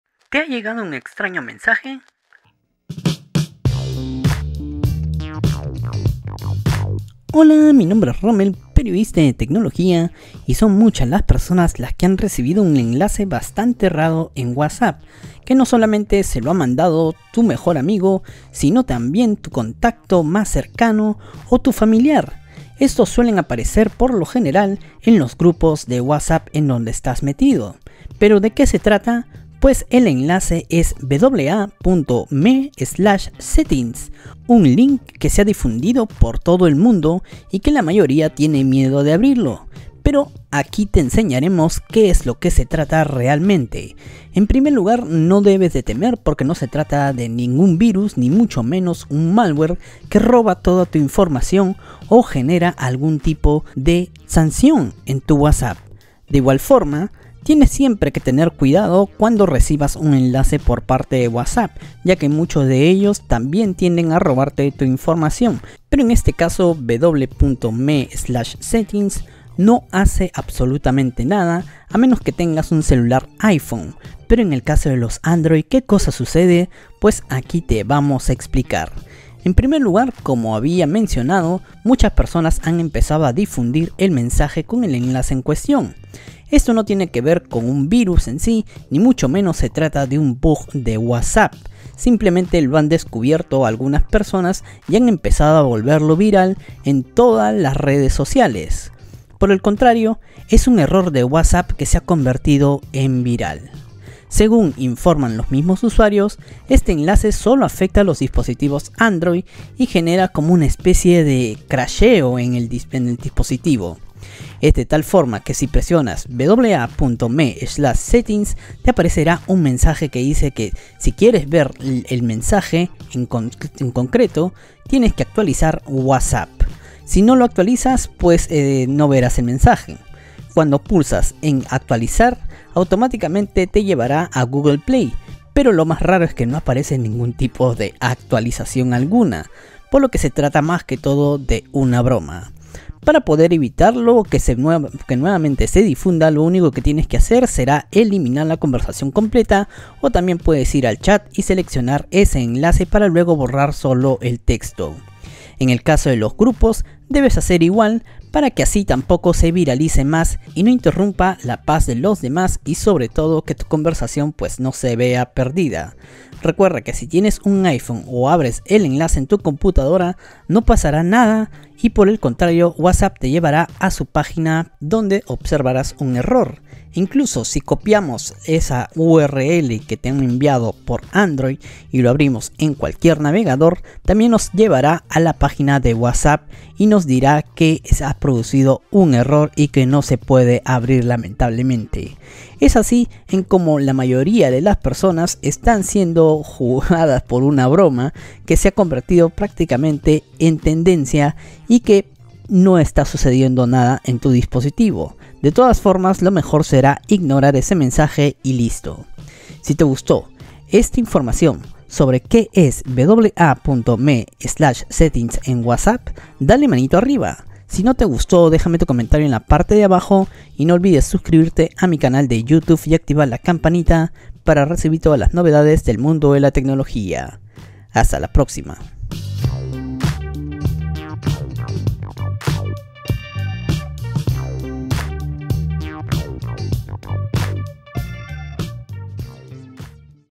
Qué es WA.ME/SETTINGS en WHATSAPP ¡Cuidado con tu smartphone!
¿Dañará tu smartphone? Conoce qué significa " y por qué se está viralizando en WhatsApp